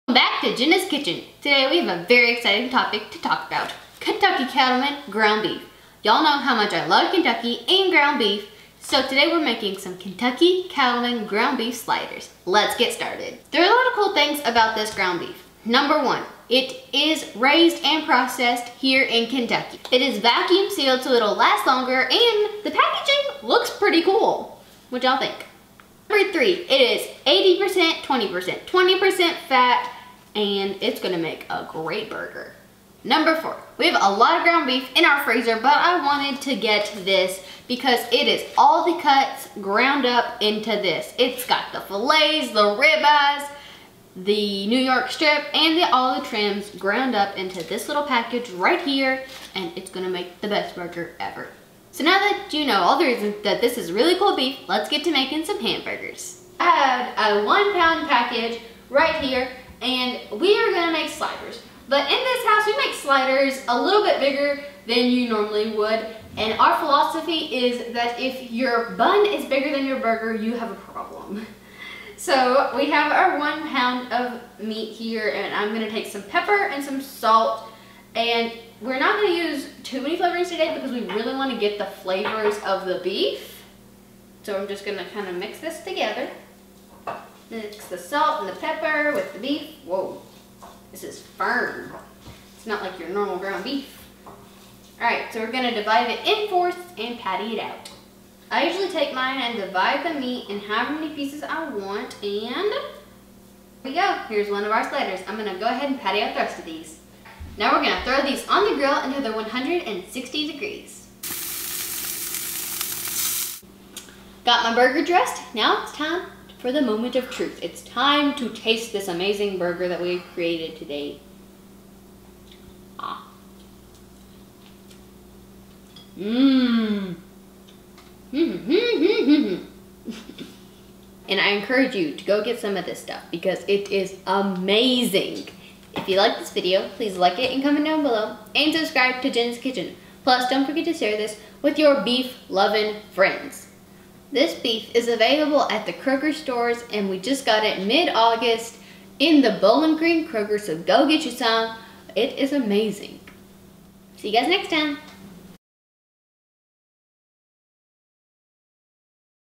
KY Cattlemen's Ground Beef
Y'all know how much I love burgers, and how much I love Kentucky and this video combines both! I use the KY Cattlemen's Ground beef to make some of the best sliders ever! If you love beef, and you love Kentucky, then this is the perfect video for you!